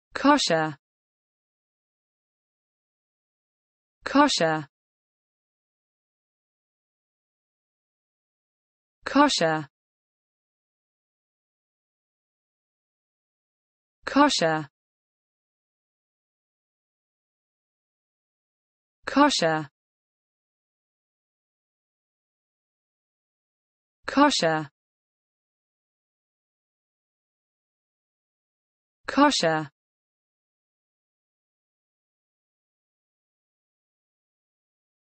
the correct pronunciation of cosher in English.
This video shows you how to pronunciation cosher in English.